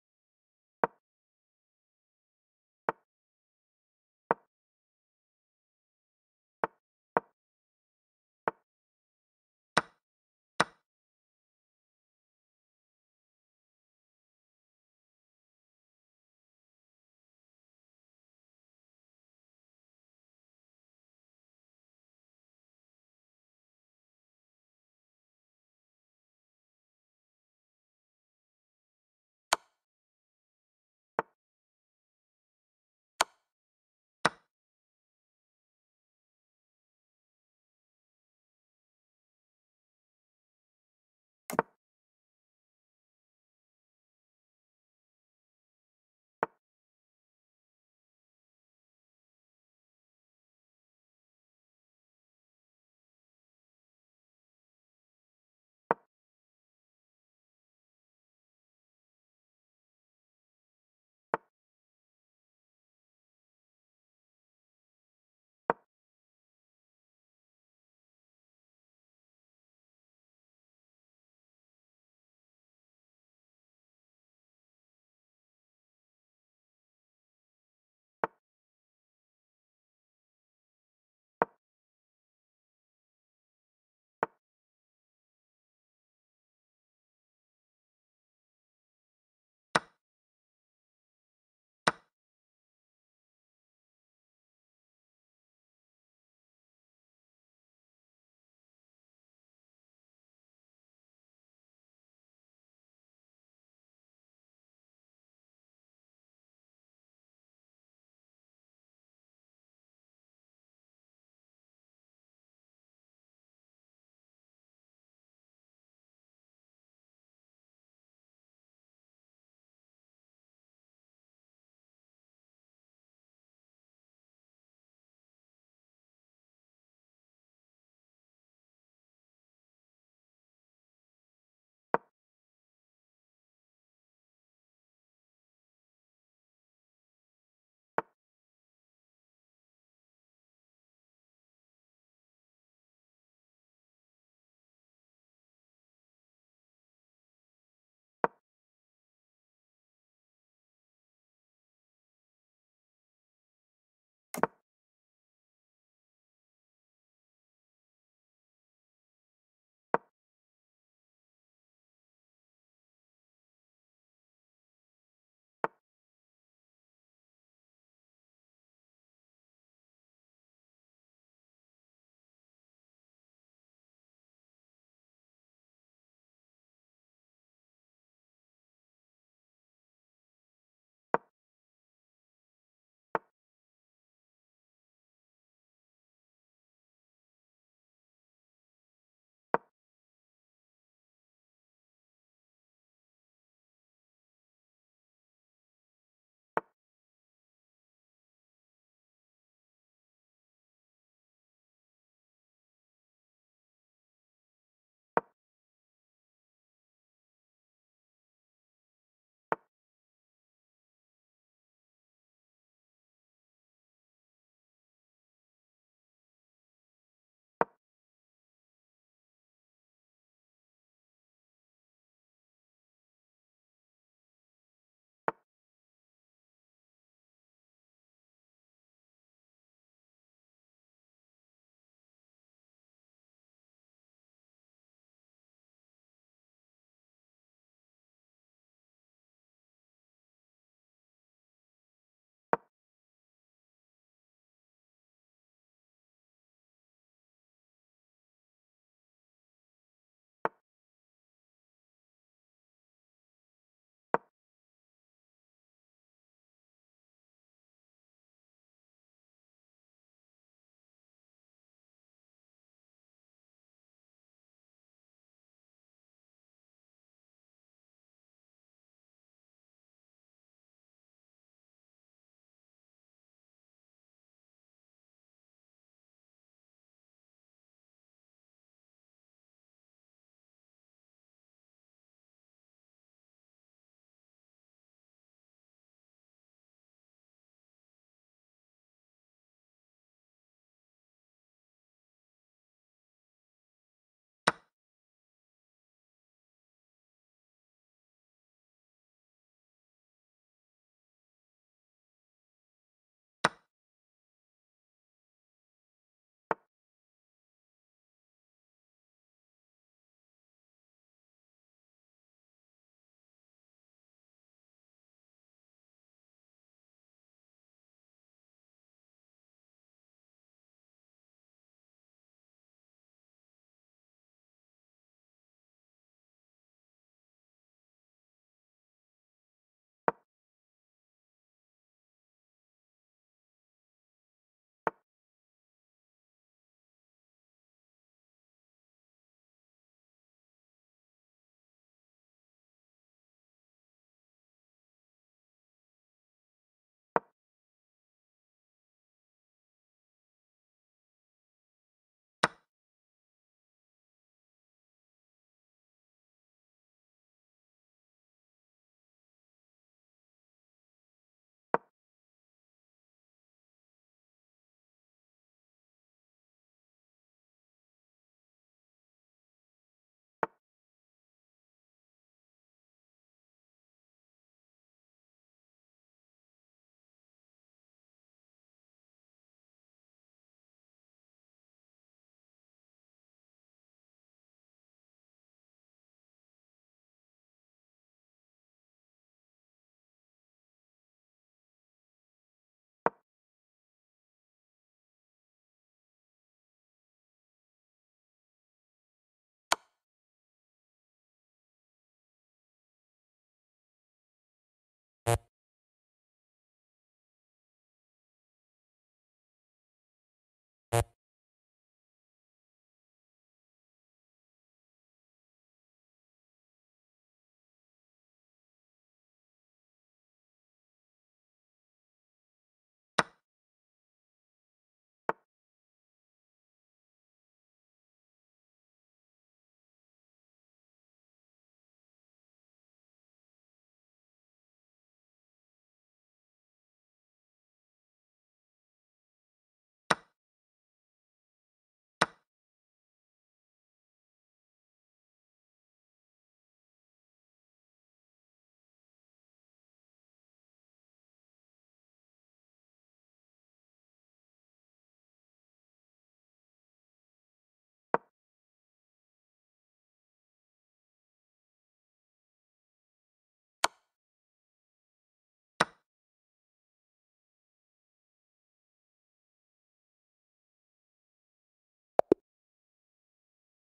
Chess Game #68
My opponent was aggressive early with the Rook, which I was able to trap and led to a further miscalculation.